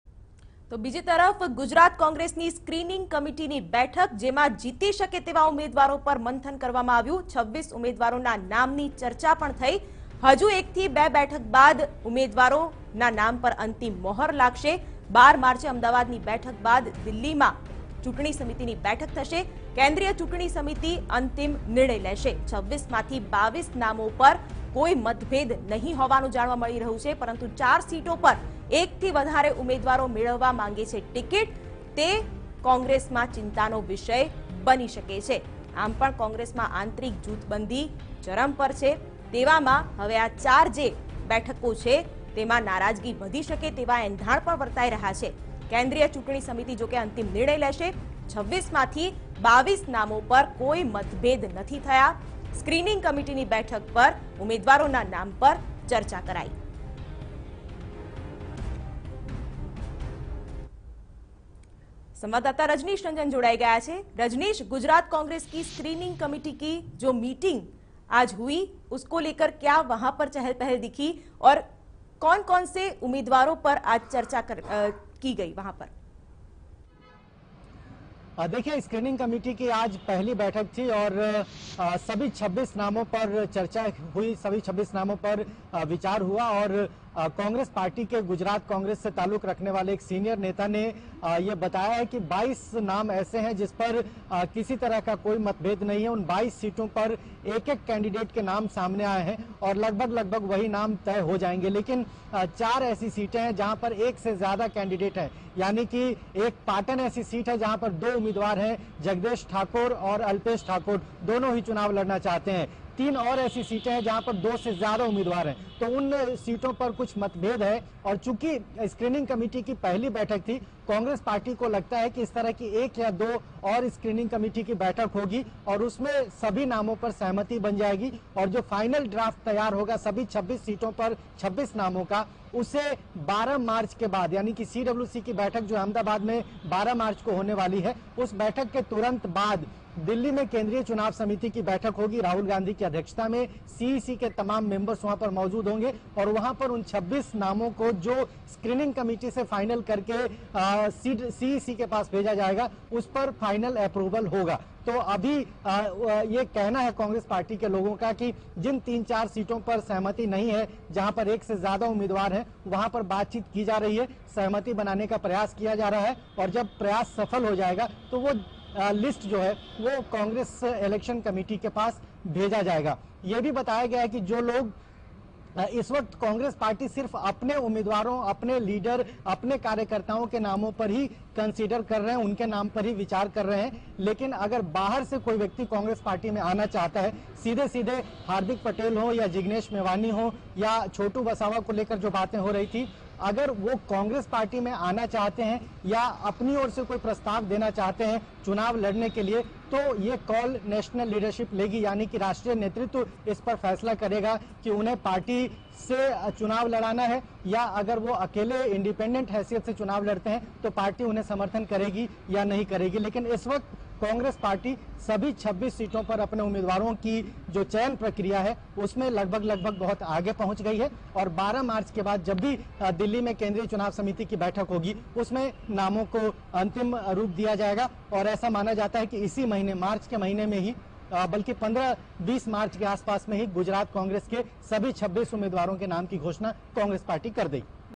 CEC will declare candidates' names for LS polls after 1-2 rounds of screening committee's meeting
Gujarat : Congress Election Committee will finalise candidates' names for LokSabha Elections 2019 after 1 or 2 more rounds of screening committee's meetings.